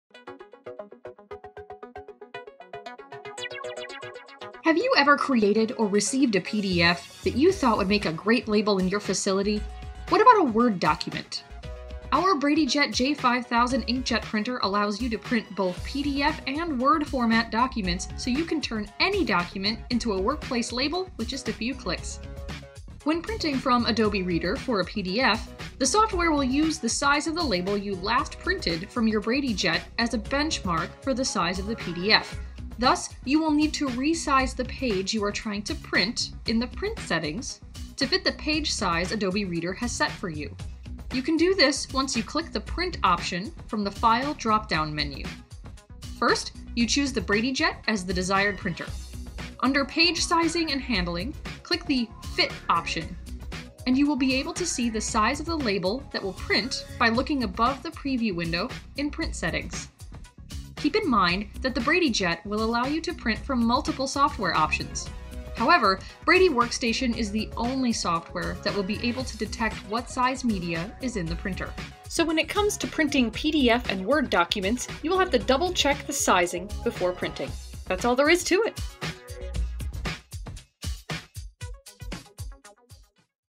BradyJet J5000 | How To Print From PDF And Microsoft Word
Did you know you can print labels from Microsoft Word and PDF on your BradyJet J5000 inkjet printer? This video walks you through the simple process.

The BradyJet J5000 Color Label Printer is an on-demand, inkjet solution for creating photo-quality color labels up to 8" wide, to create a safer, more efficient workplace.